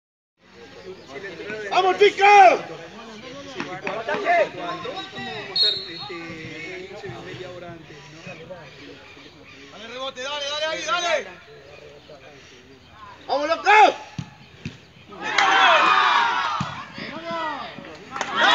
Penal atajado por Alan Velázquez a Jehiel Romitti
Unión de Santo Domingo - Alumni Laguna Paiva